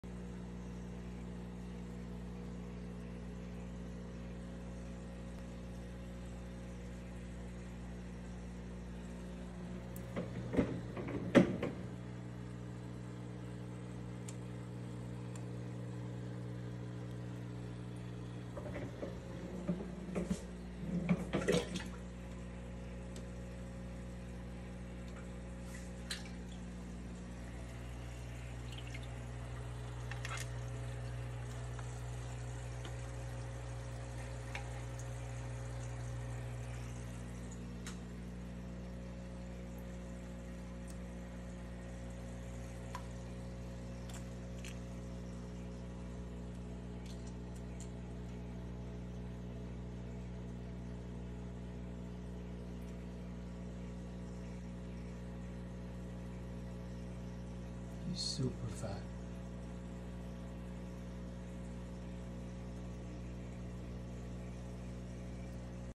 Green Spotted Puffer eating juicy Ramshorn Snail for dinner
Nothing beats a healthy, organic, fresh, crunchy and juicy snail.